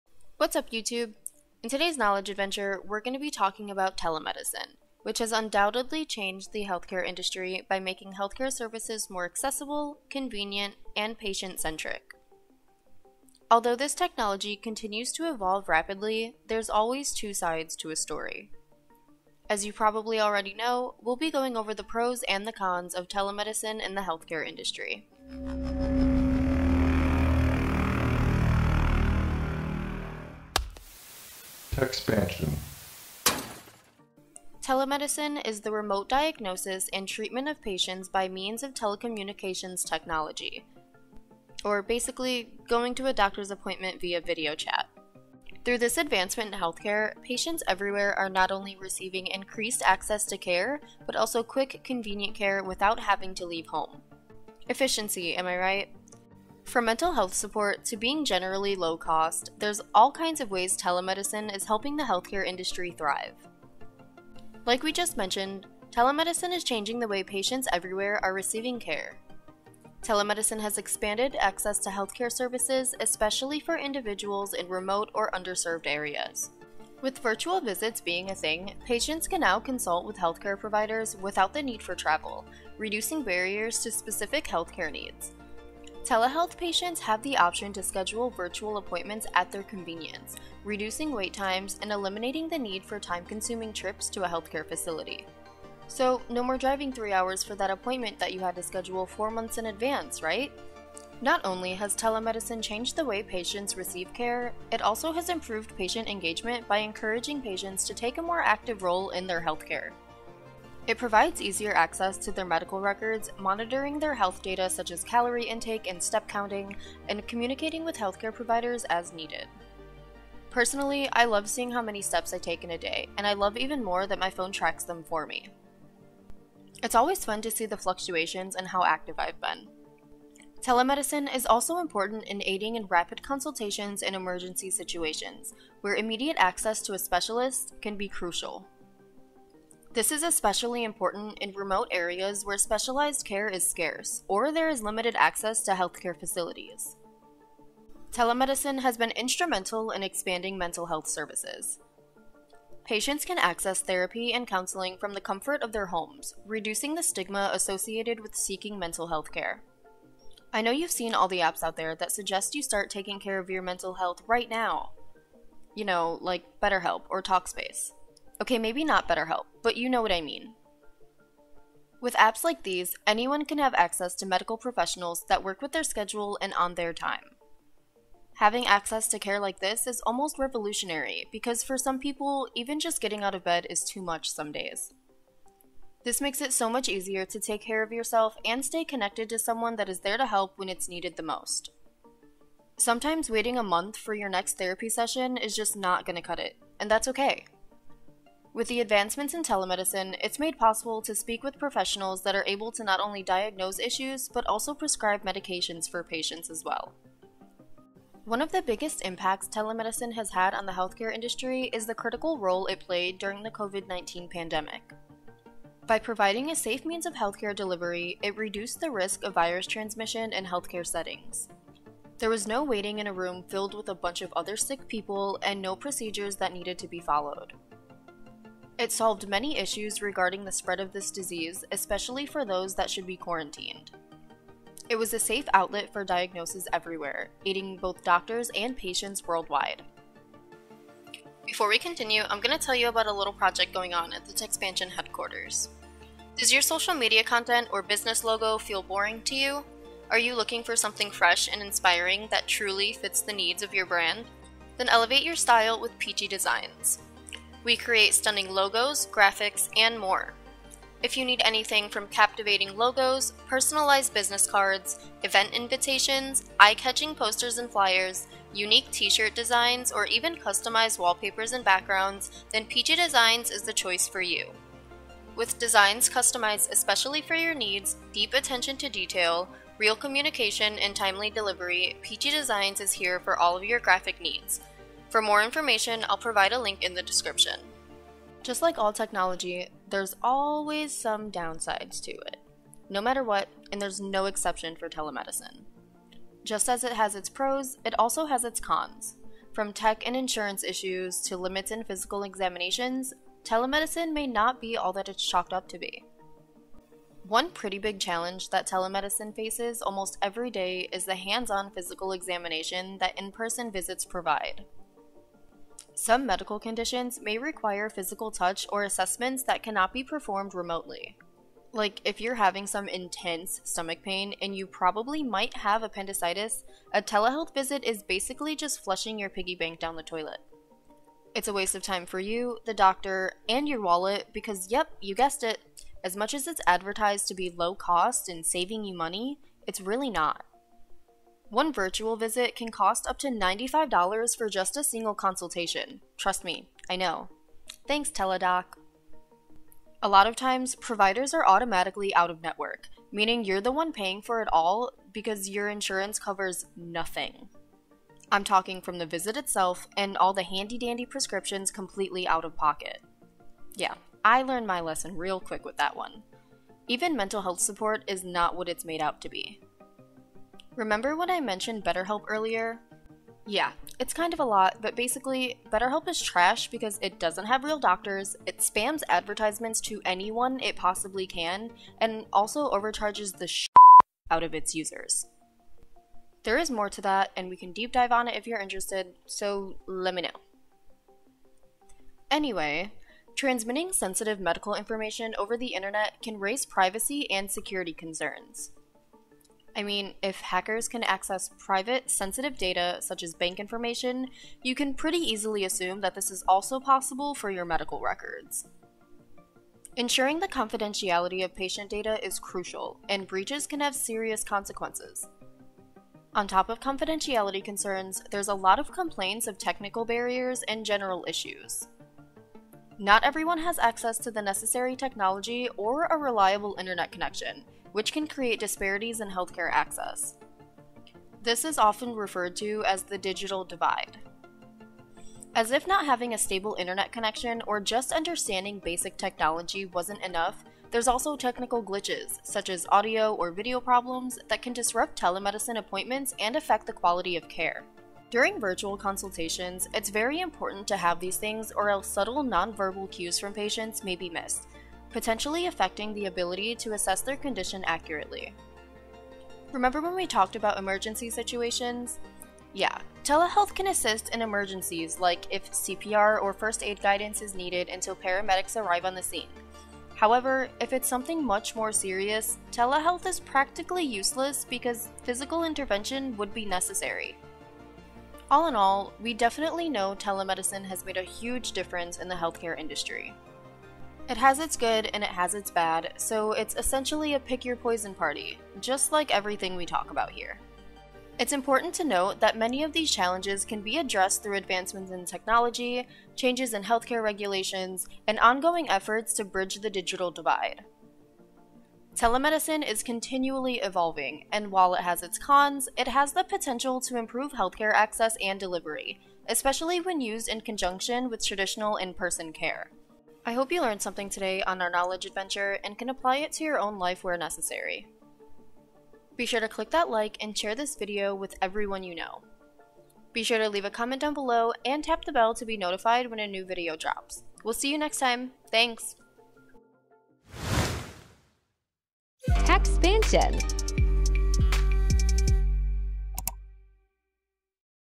Telemedicine Is Changing the Healthcare Industry!
*Sorry ahead of time guys, this was a backlogged video.*
In this video we talk about how telemedicine services may be a good or bad thing to get behind. Does it really help?
Telemedicine is relatively recent technology, so lets learn more.
TecXpansion Links:
TecXpansion Facebook:

Collabs:
Voiced by:
FurMama Peach -Youtube